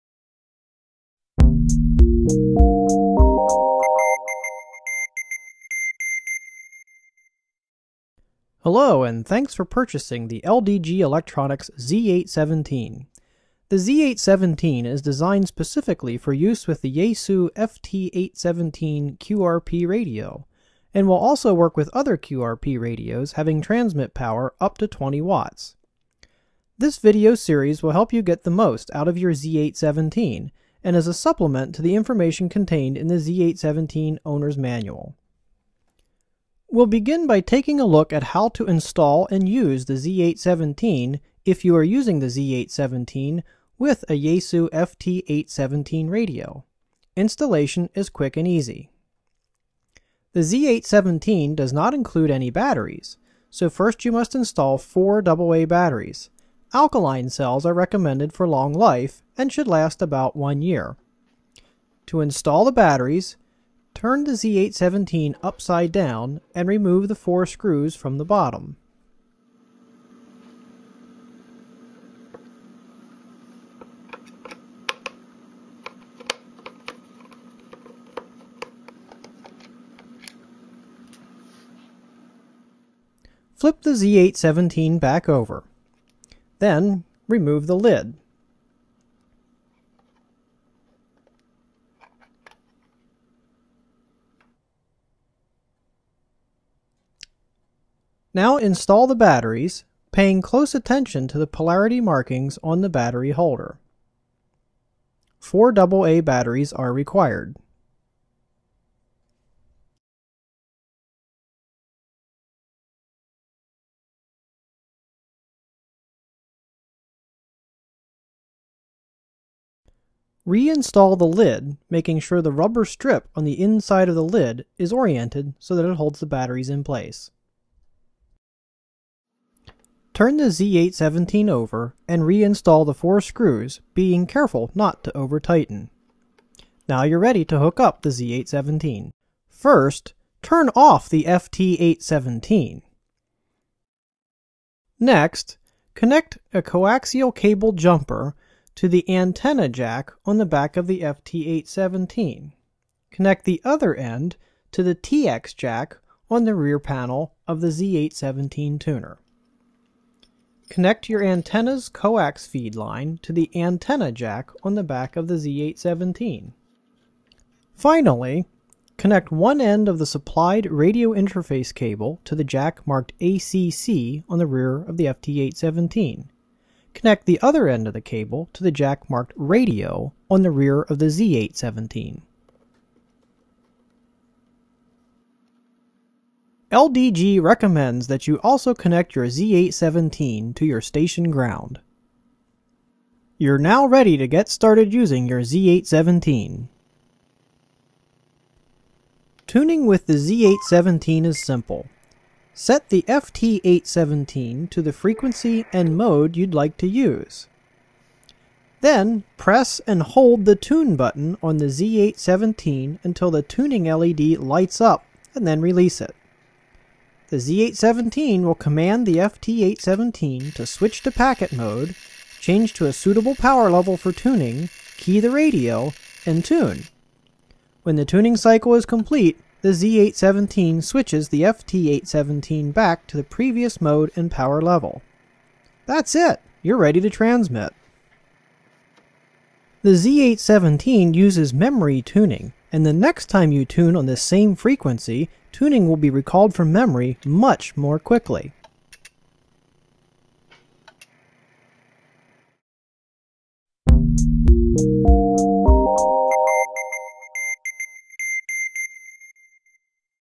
Z-817 Tuner Part 1
Part 1 of 2. LDG Electronics Z-817 tuner video supplement.
In Part 1, we discuss installation and usage of the Z-817 tuner with the Yaesu FT-817 QRP radio.